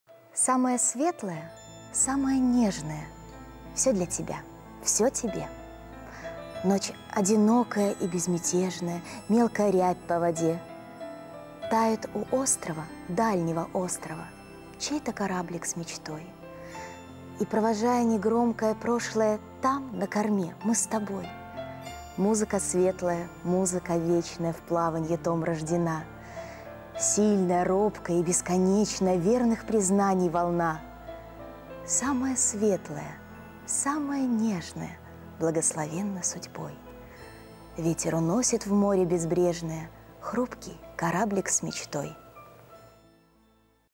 Авторское стихотворение «Кораблик с мечтой»
Ольга Колесникова, певица, артистка театра эстрады

Самое светлое, самое нежное -
Всё для тебя, всё тебе…
Ночь одинокая и безмятежная…
Мелкая рябь по воде.
Тает у острова, дальнего острова
Чей-то кораблик с мечтой…
И, провожая негромкое прошлое,
Там, на корме, мы с тобой.
Музыка светлая, музыка вечная
В плаванье том рождена.
Сильная, робкая и бесконечная
Верных признаний волна.
Благословенно судьбой.
Ветер уносит в море безбрежное
Хрупкий кораблик с мечтой.